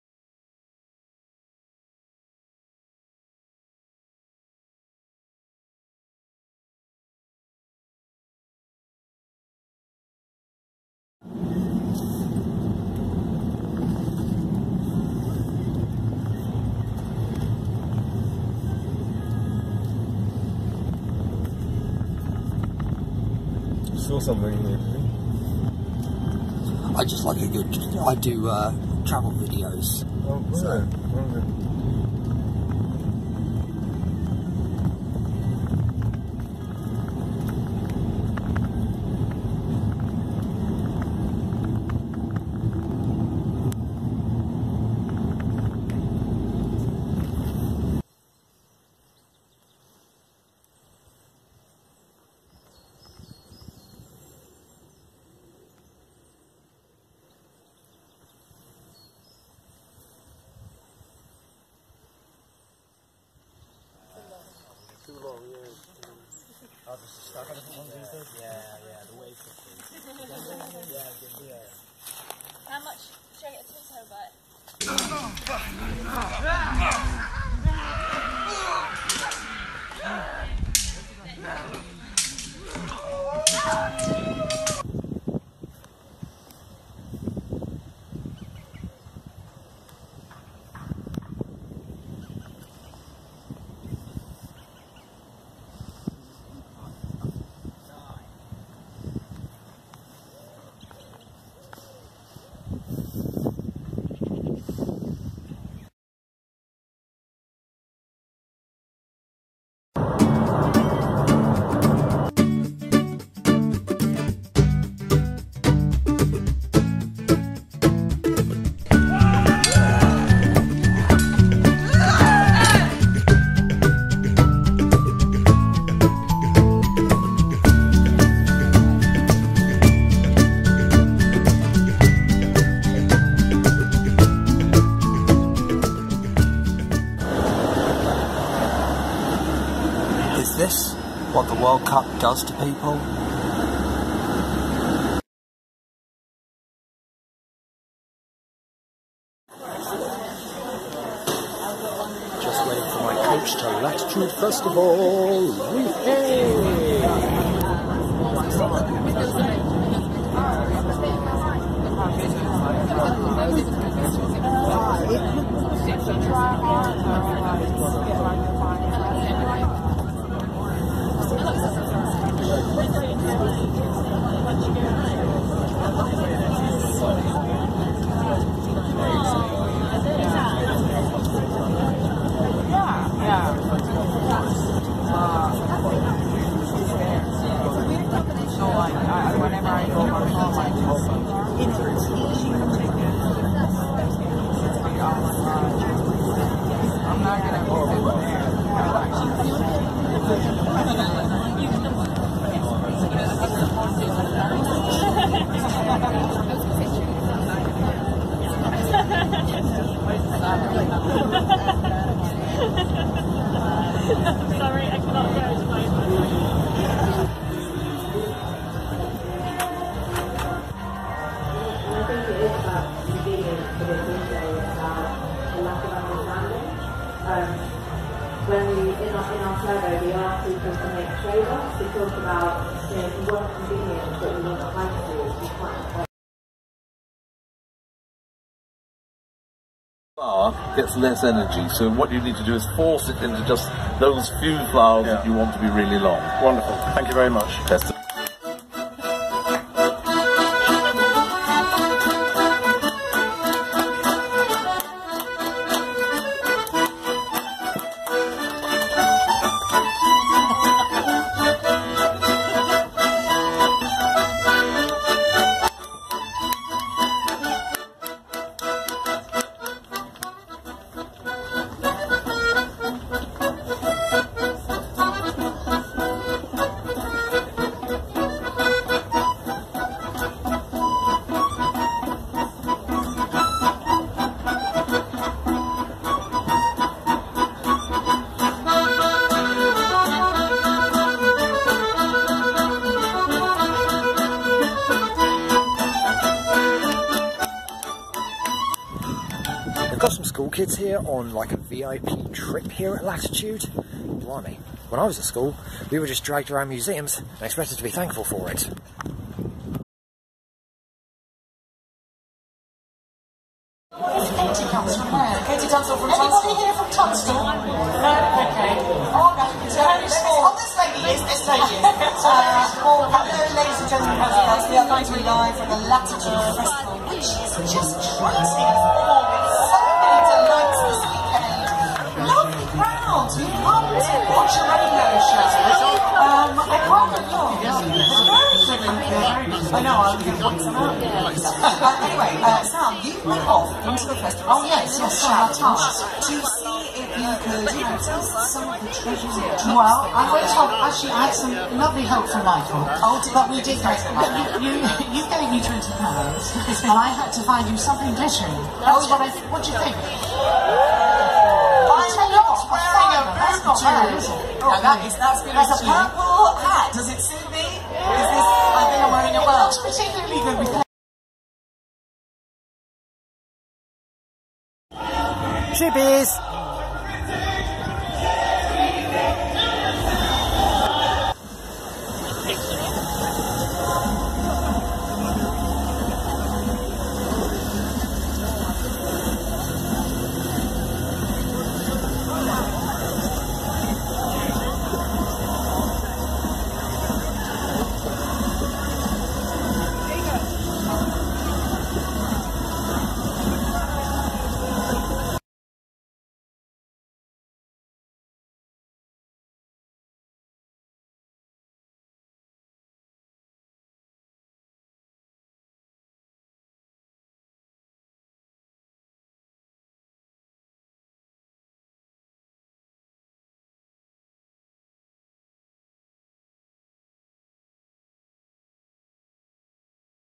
Films and Festivals Latitude & Beserker Death fields
Travel video featuring Nicks adventures out and about in london and all different areas today its  Films and Festivals Latitude & Beserker Death fields